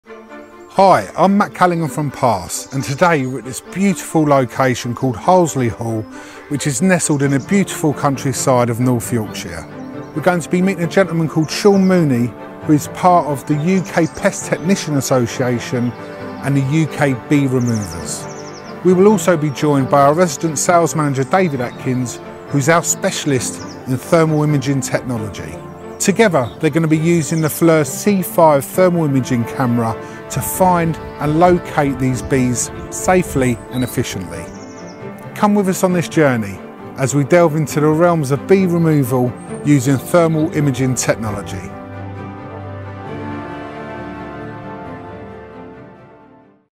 Discovering Hidden Bee Hives with Thermal Tech! | FLIR C5 Thermal Imaging Camera
🔍 Unveil the secret lives of bees with the power of thermal imaging technology! 🌡️🐝 Join us on this captivating adventure as we use the FLIR C5 thermal imaging camera to locate hidden bee hives. Witness the remarkable capabilities of this innovative tool as we delve into the world of beekeeping like never before. 🎥✨ Don't miss out on this mesmerizing discovery!

Full story coming soon.